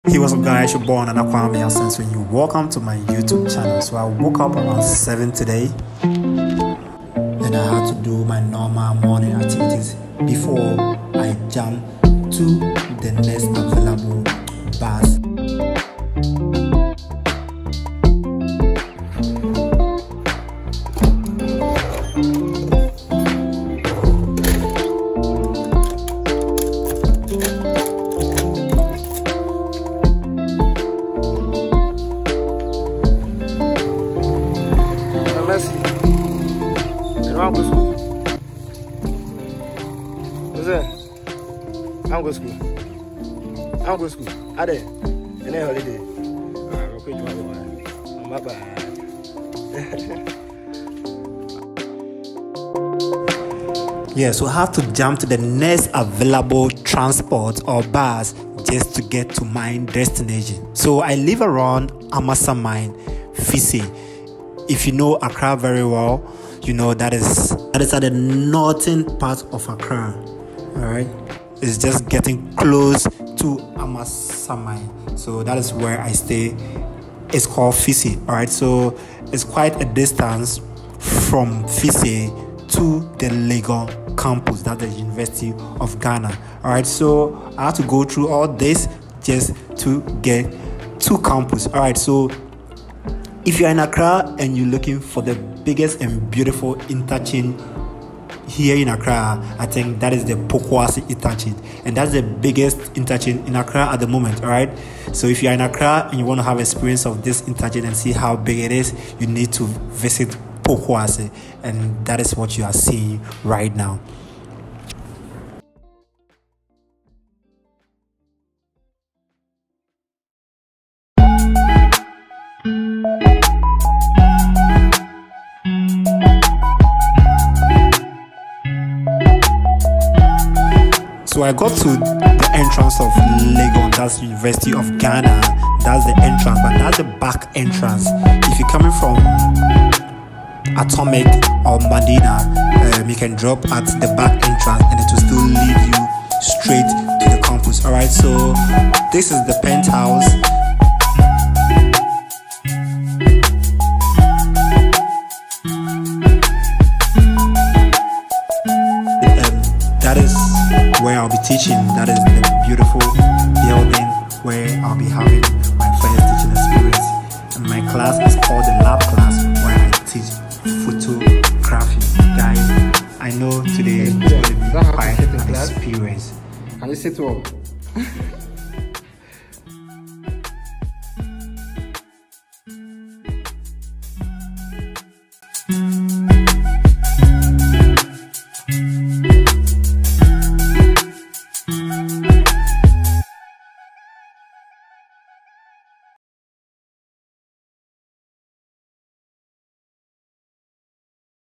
My first teaching experience in Legon (University of Ghana)
A video of how I moved from Fise , a small town around Amasaman to the university of Ghana campus to teach photography for the first time. Even though I have been practicing photography and videography for years now..